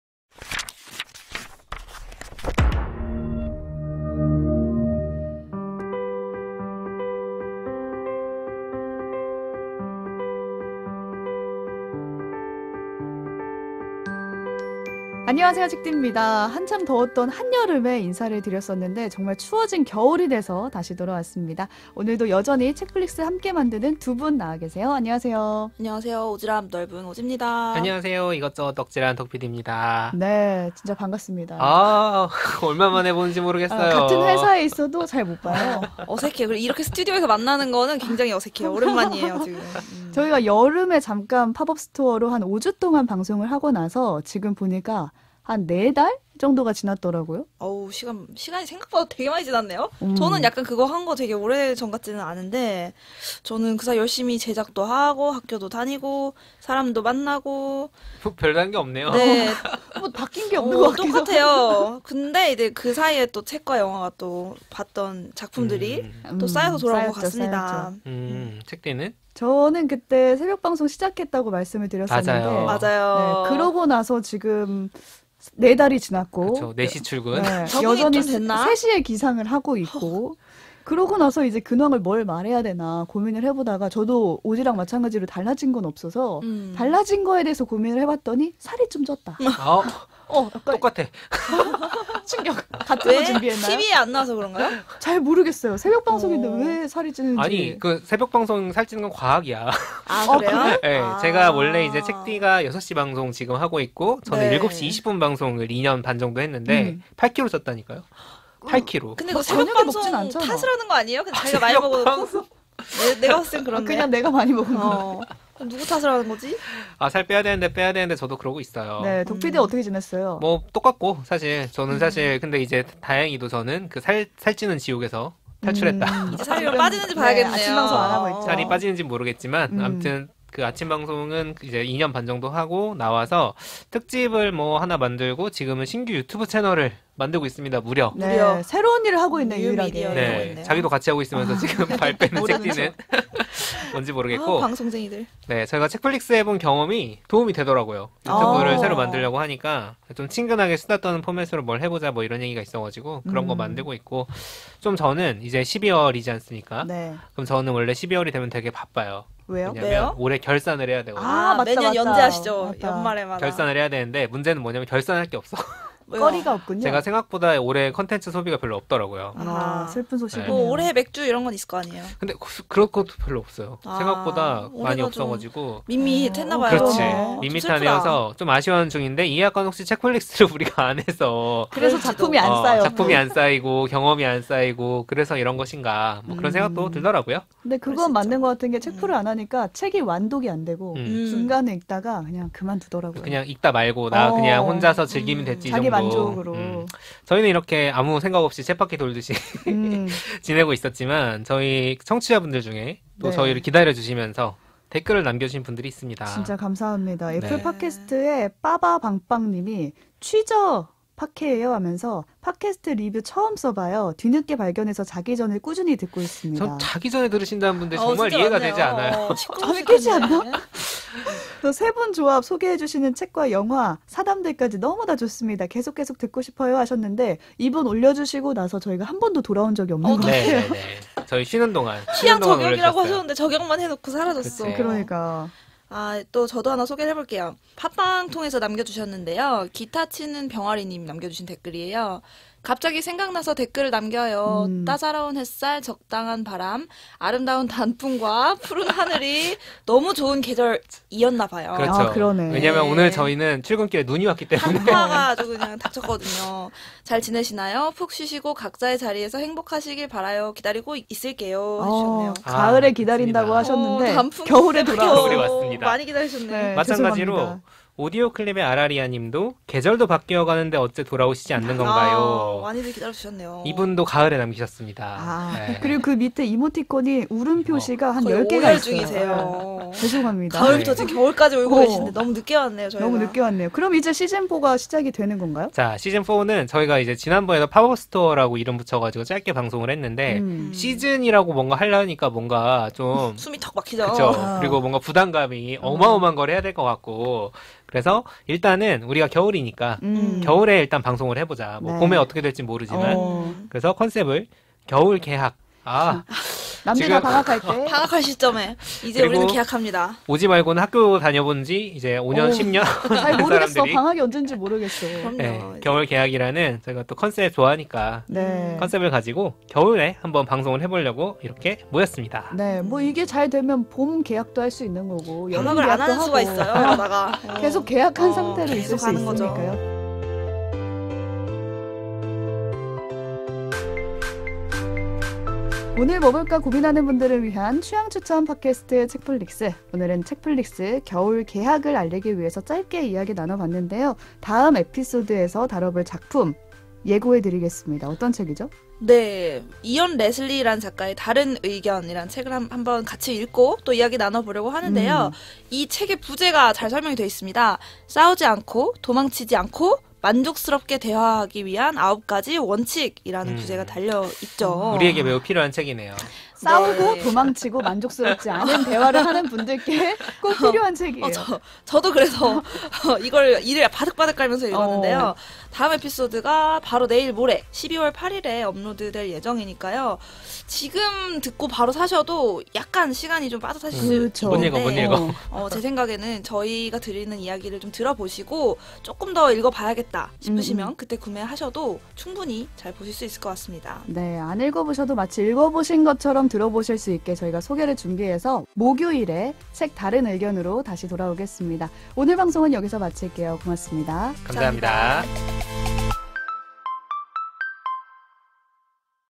※ 책플릭스 겨울개학 ※
[공지] 책플릭스 겨울개학
- 우리와 여러분의 근황
- 시즌4 언제 하나요
- '헤어질 결심' Coming Soon